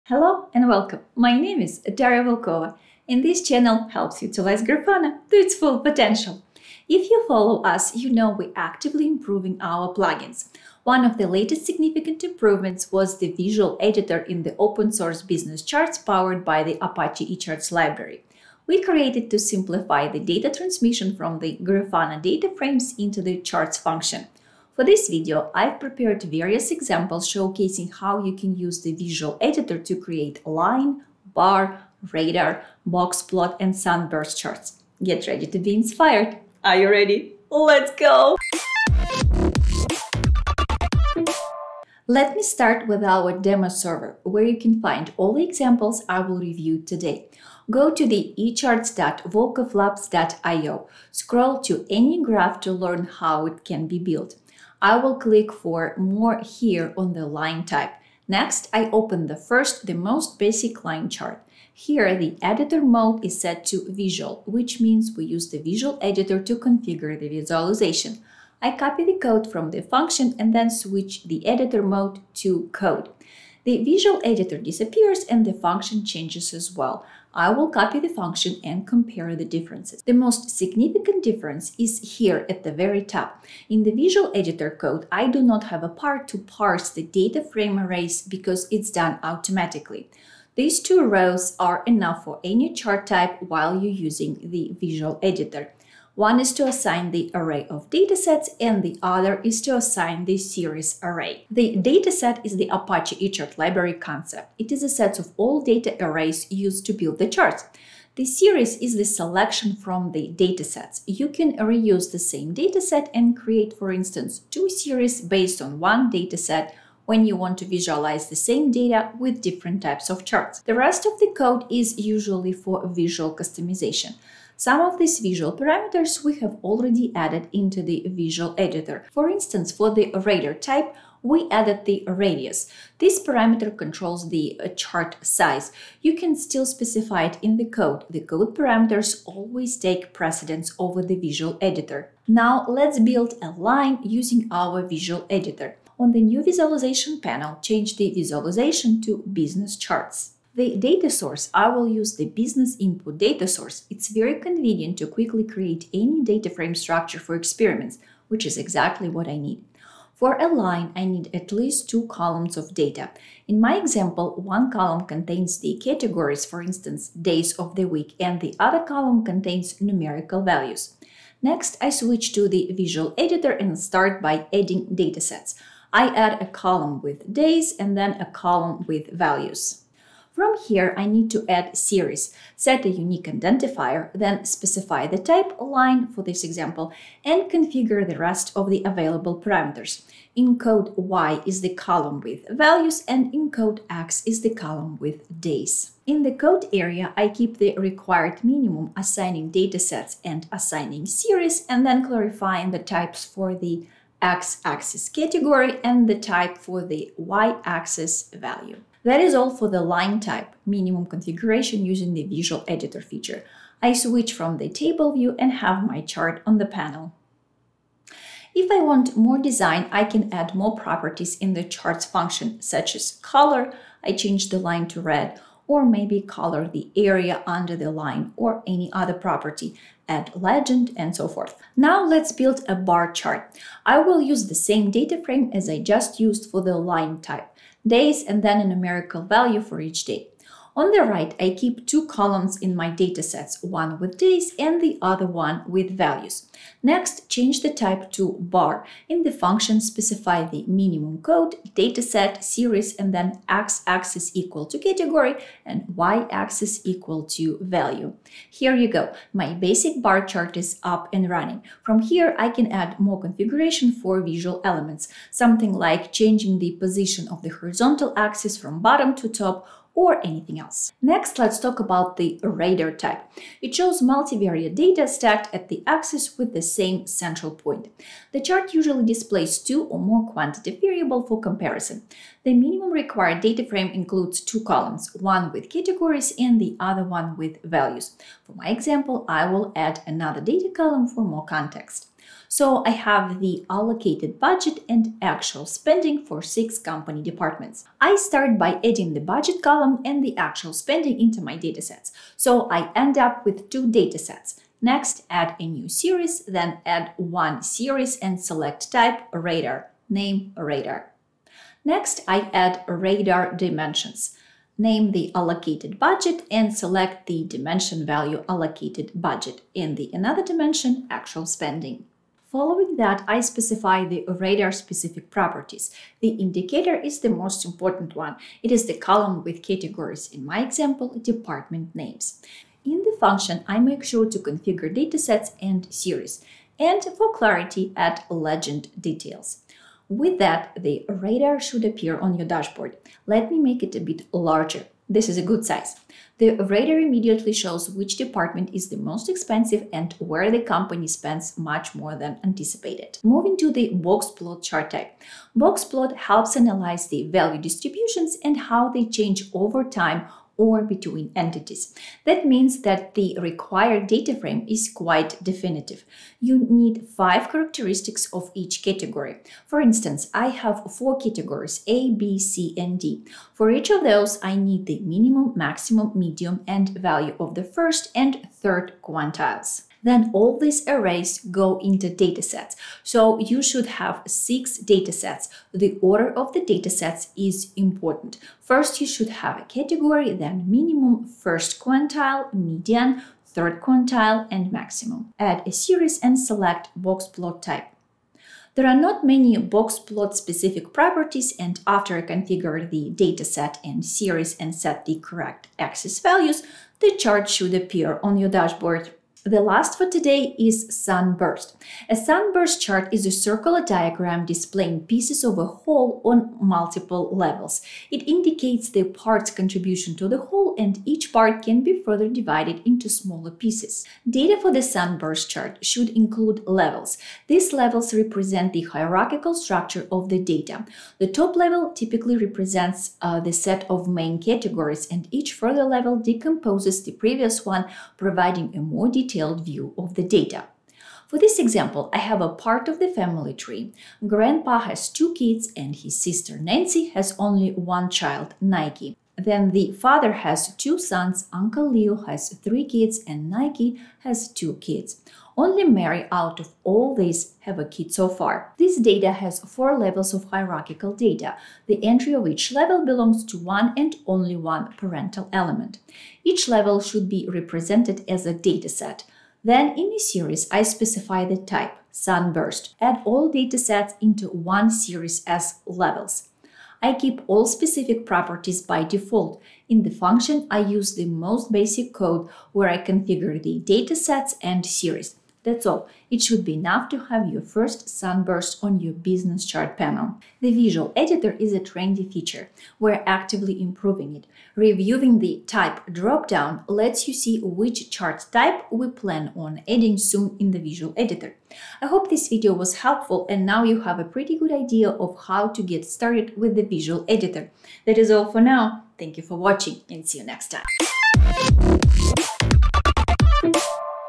Visual Charts Editor tutorial | Business Charts 6.4.0 for Grafana
We created the Visual Editor to simplify working with the Grafana data frames and facilitate intuitive data transmission into the Charts function.  This video provides line, bar, radar, boxplot, and sunburst types tutorials.

CHAPTERS
0:00 Intro
0:41 Demo Server echarts.volkovlabs.io
0:58 Compare the Visual Editor code with regular code
2:21 Build a line with the Visual Editor
3:51 Build a bar with the Visual Editor
4:31 Build a radar with the Visual Editor
5:52 Build a boxplot with the Visual Editor
6:49 Build a sunburst with the Visual Editor